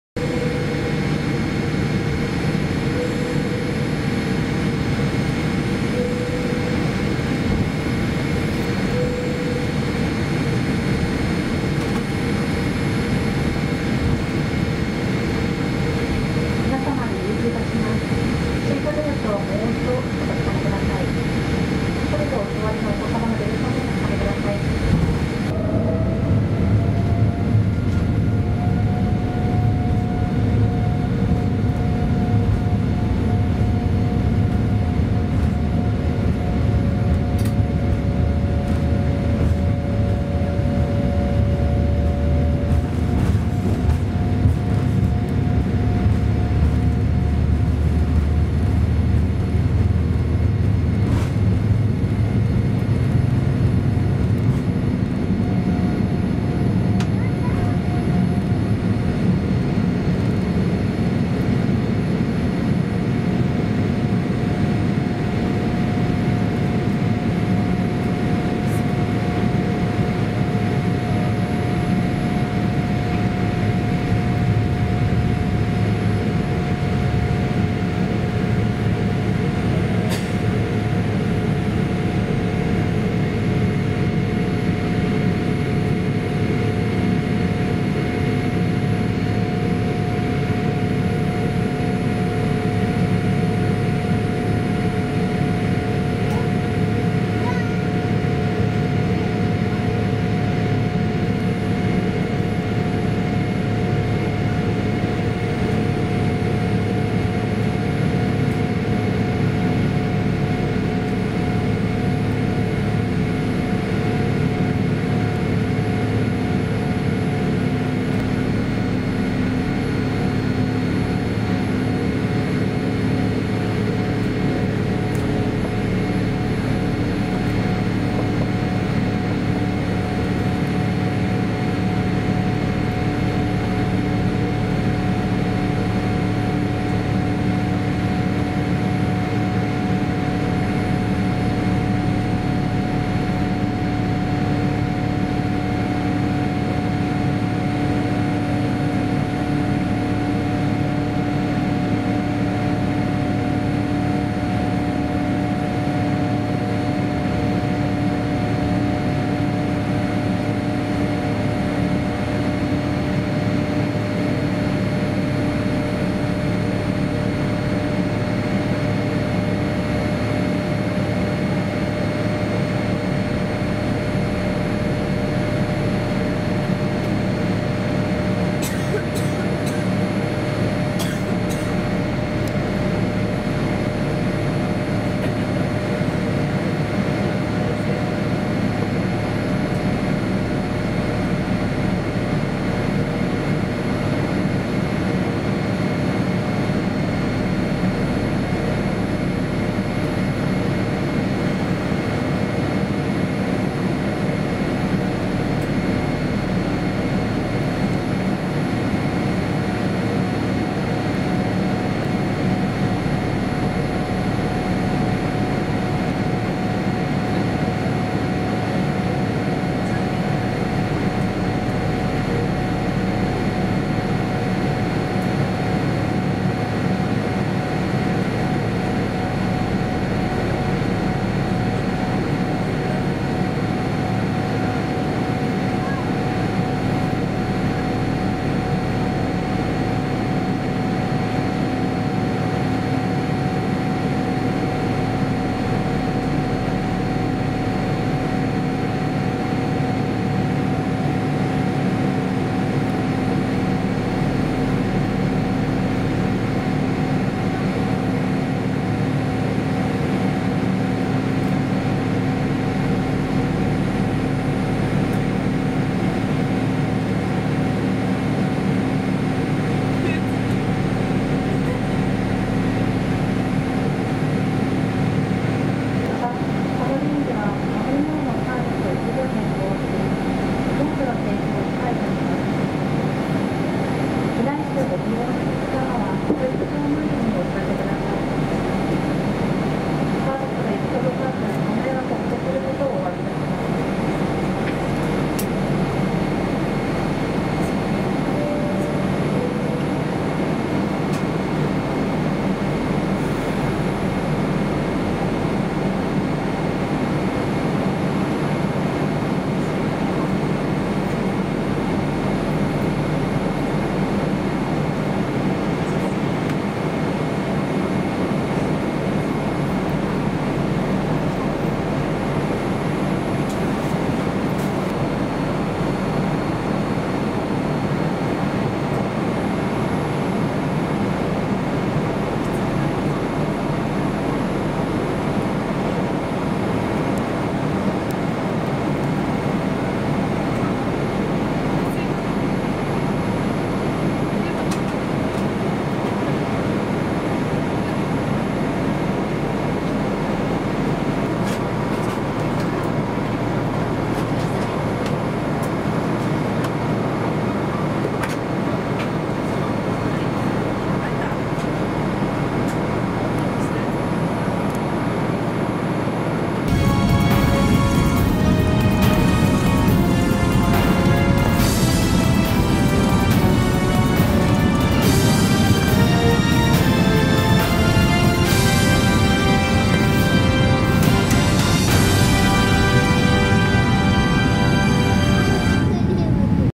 早朝の羽田空港D滑走路離陸～大島空港／ANA993羽田空港→那覇空港
2021年7月25日撮影、ANA993便羽田空港→那覇空港B787-8(JA809A)前方左側プレミアムクラスより撮影した早朝の羽田空港D滑走路離陸～大島空港映像です。

【撮影機材】
Panasonic 4K VZX990M 64GB HC-VZX990M-W
4K ビデオカメラ VZX992M

Everyday Update!